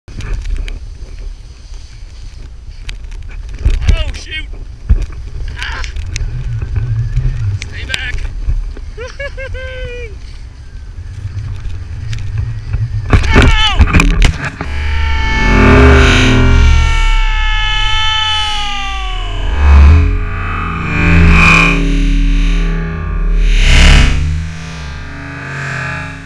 DMac Crash at Loveland Ski Area - GoPro
104.3FM The Fan's DMac crashes down Marmot at Loveland Ski Area. GoPro